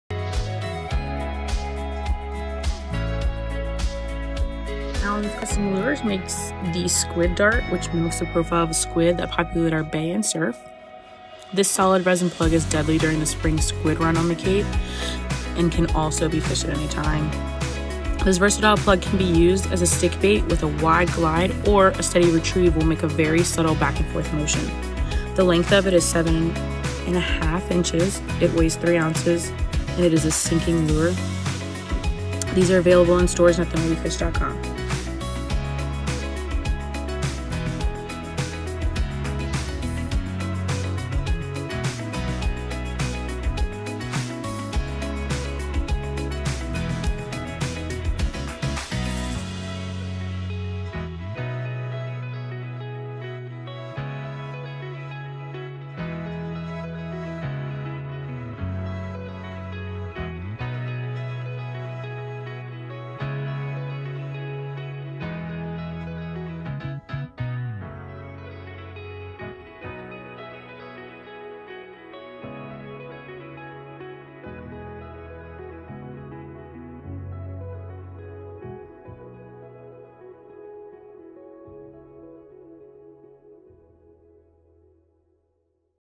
Alans Custom Lures Squid Dart - Underwater Footage!
The Squid Dart  mimics the profile of the squid that frequent our bays and surf.  This solid resin plug is deadly during the spring squid run on the Cape but can be fished at any time.

A steady retrieve results in very subtle back and forth. It can be used as a stickbait with a wide glide.  A very versatile plug.

Length: 7.5"
Weight: 3.0 oz
Type: Sinking